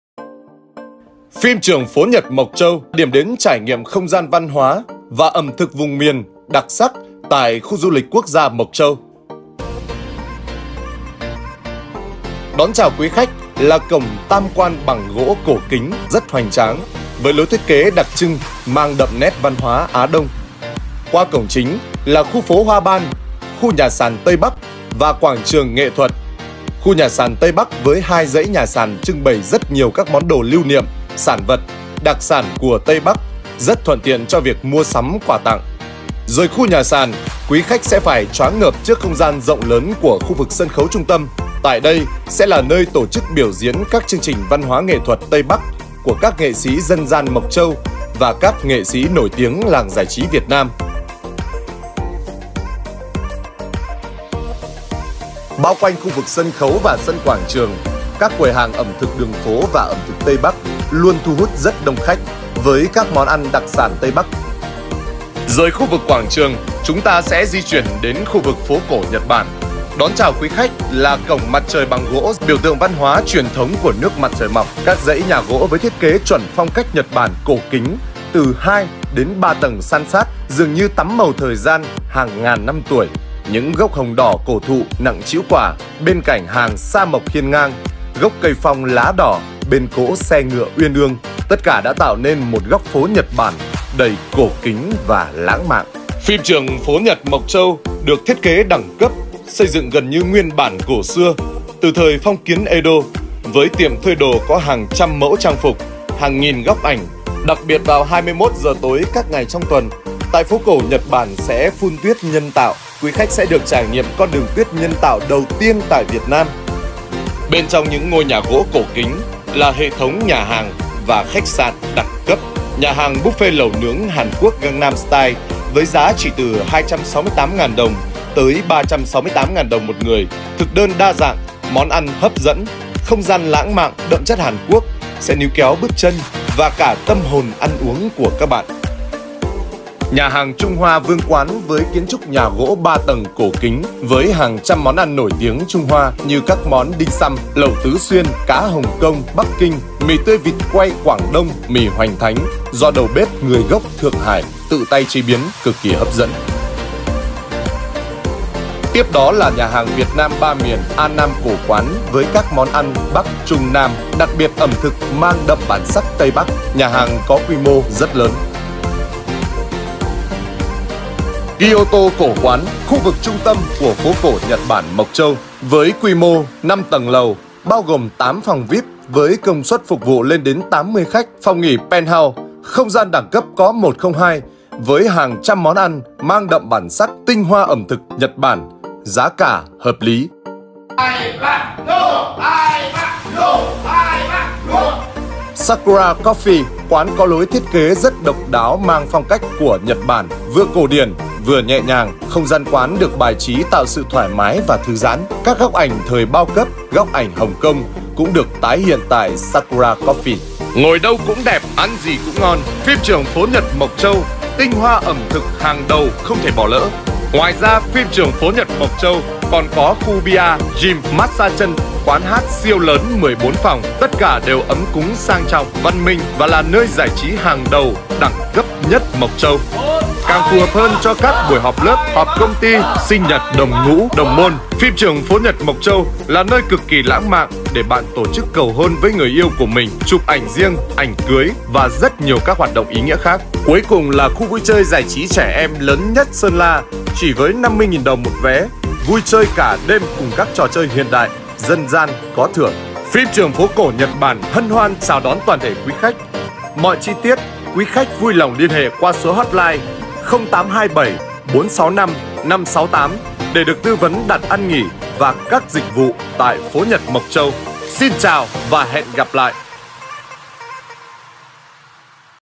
PHIM TRƯỜNG PHỐ NHẬT MỘC CHÂU - ĐIỂN ĐẾN HÀNG ĐẦU I Phim trường Phố đi bộ Mộc Châu.
Phim trường phố Nhật Mộc Châu điểm đến trải nghiệm không gian văn hóa, ẩm thực vùng miền đặc sắc tại Khu du lịch quốc gia Mộc Châu. Xin mời các bạn hãy cùng tìm hiểu những điều đặc biệt về Phim trường Phố nhật Mộc Châu.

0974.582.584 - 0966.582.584 để được tư vấn đặt ăn nghỉ và các dịch vụ tại Phố Nhật Mộc Châu.